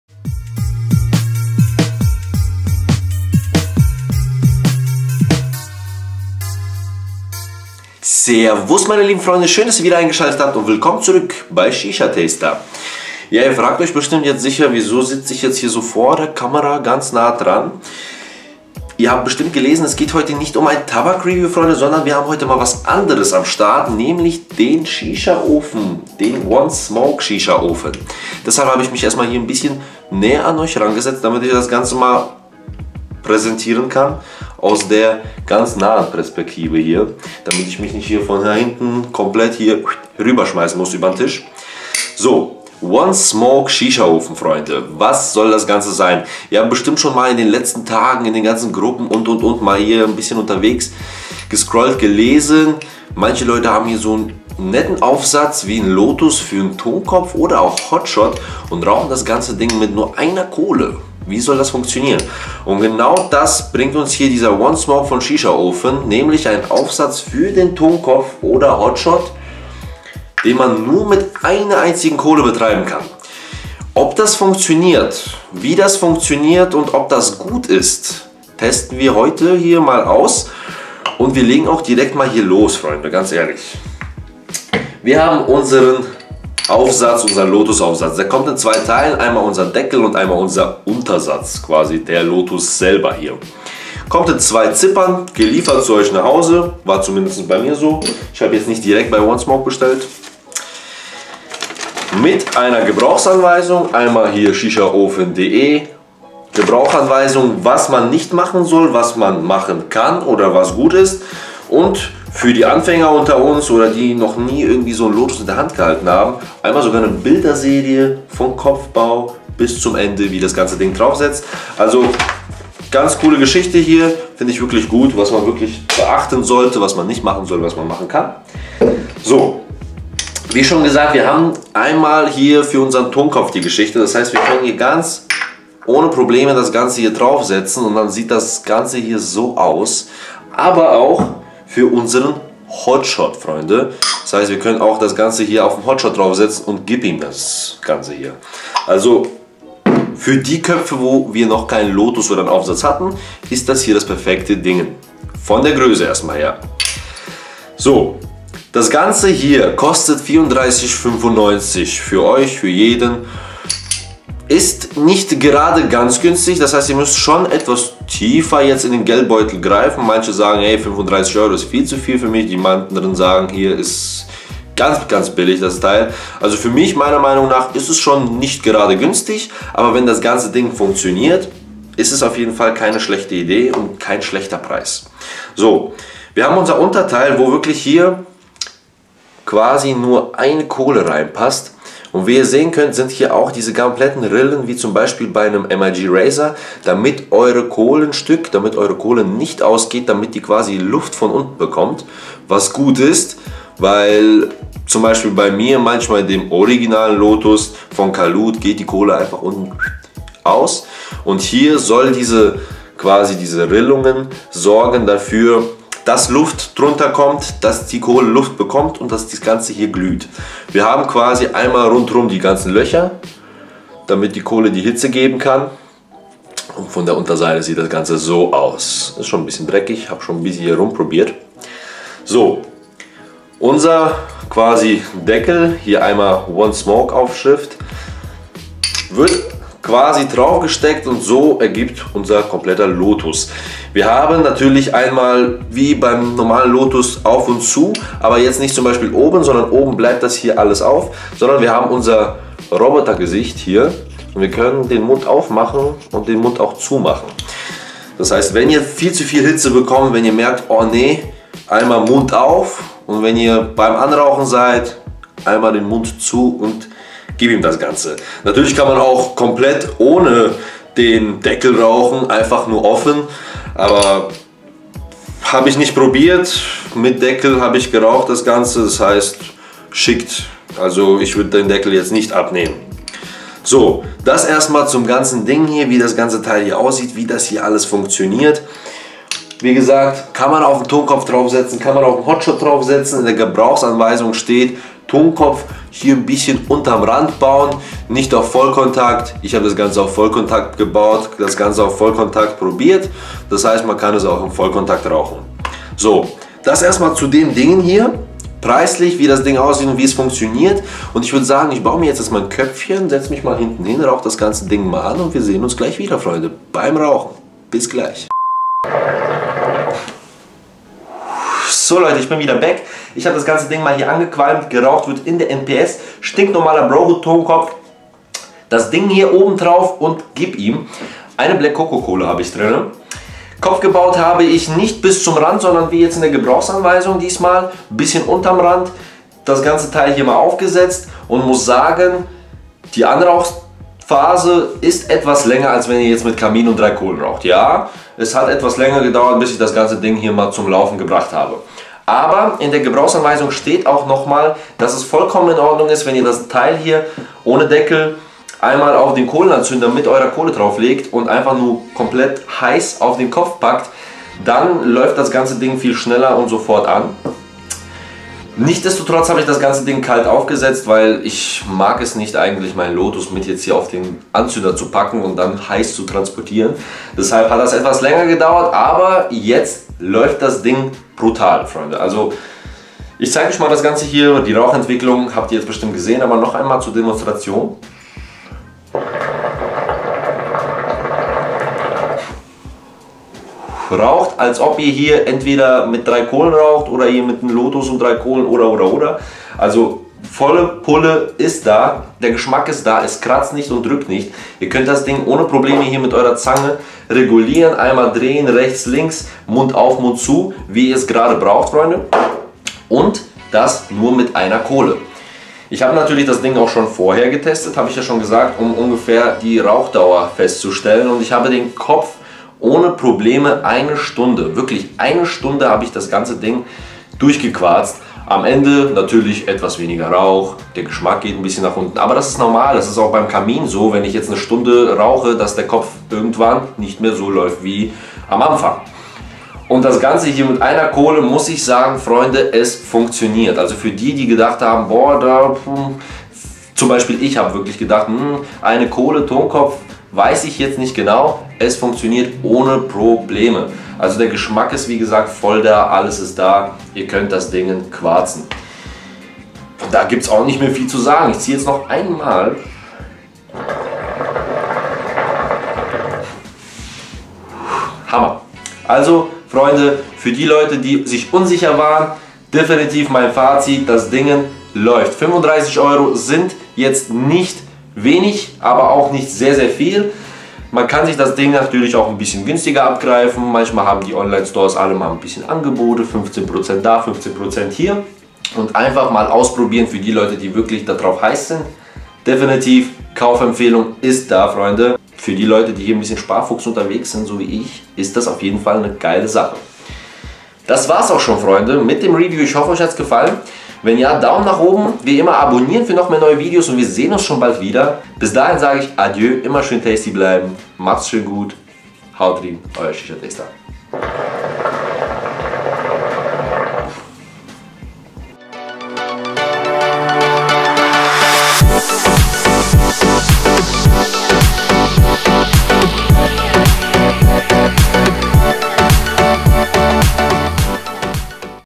ONE SMOKE | RAUCHEN MIT EINER KOHLE ??!! | SHISHA TASTER
Stellt er jeden Aufsatz in den Schatten ?

Heute mal eine Produktvorstellung Freunde !

Der OneSmoke von Shisha Ofen, erhältlich ist das ganze bei Shishaofen oder Smoking-Shisha.de und verwandelt die Hütte mit EINER KOHLE zur Räucherkammer !

Aber schaut selbst !